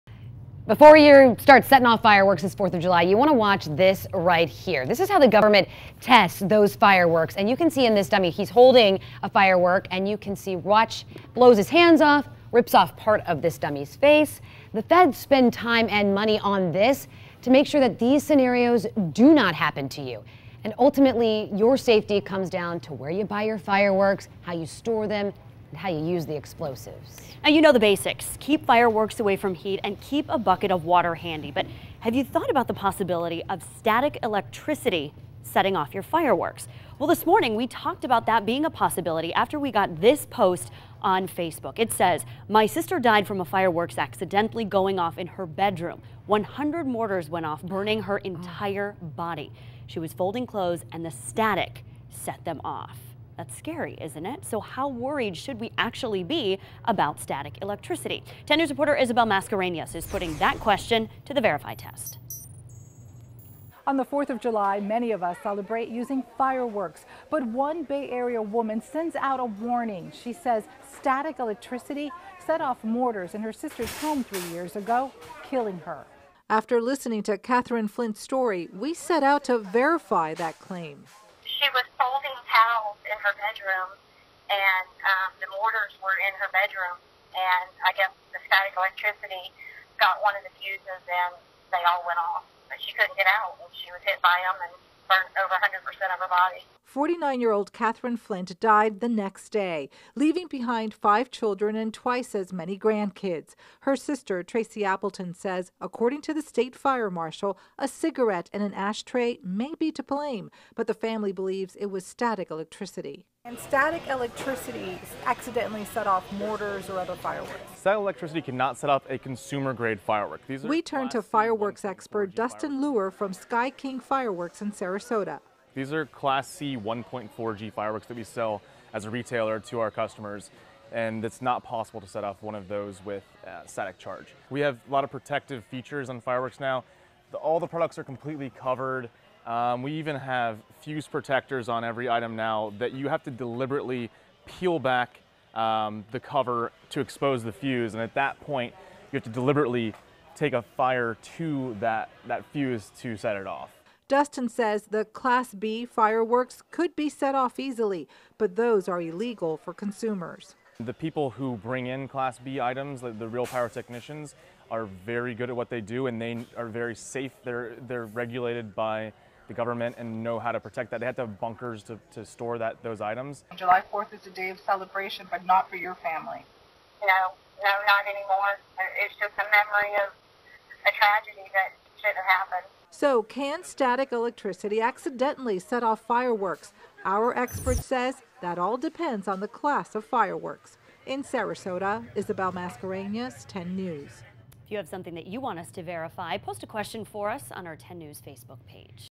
VERIFY: Can static electricity set off fireworks?
A local woman blames static electricity for setting off fireworks, killing her sister. We test that claim.